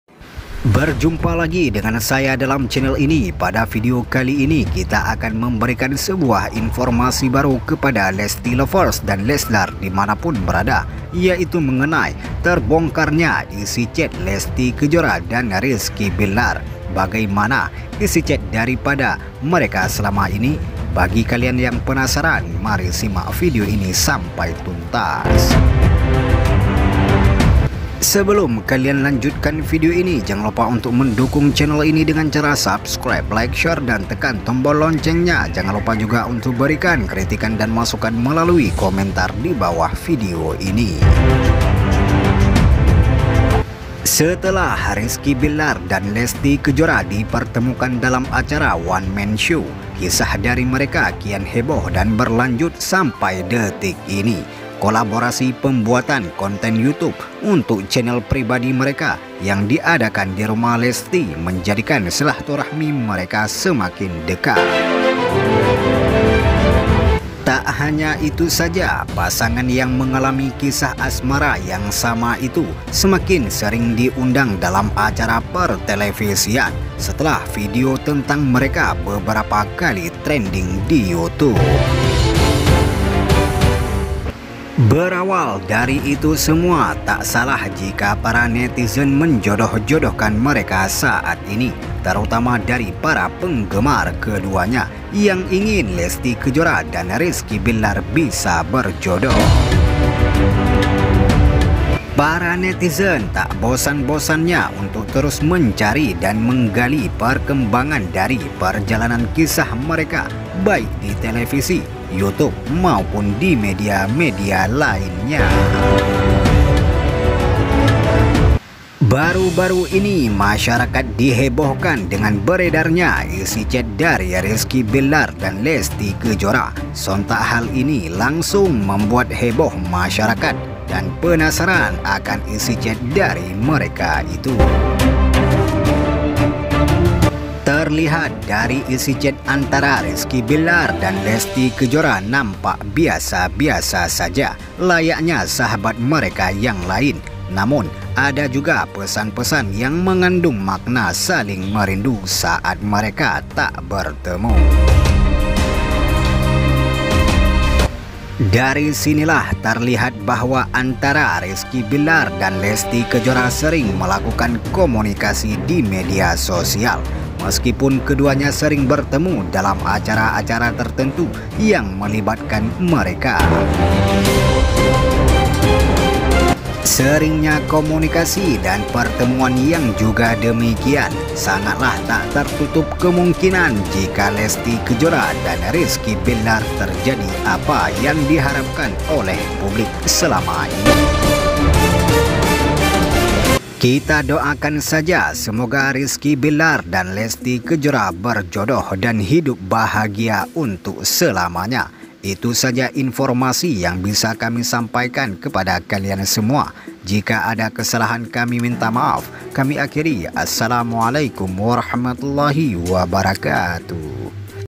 TAK DISOROT KAMERA, BONGKAR ISI CHAT RIZKY BILLAR DAN LESTY DIMOMENT KONSER 17SAN INDOSIAR
TERBONGKAR ISI CHAT RIZKY BILLAR DAN LESTY DIMOMENT KONSER 17SAN INDOSIAR.